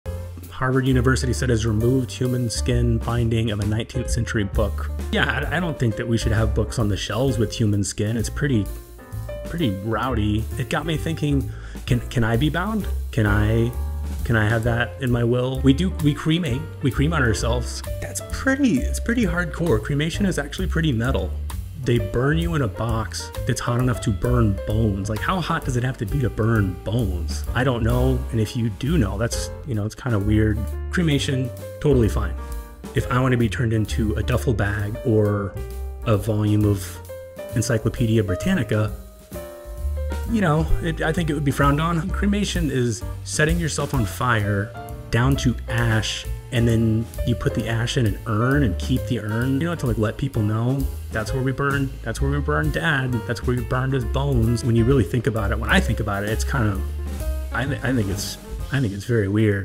Cremation is Metal, Can I Be a Book? TBT 12
Harvard had a book bound in human skin and it got me thinking about how cremation is pretty metal but we act like it's really casual. Can I be a book? Or a nice jacket?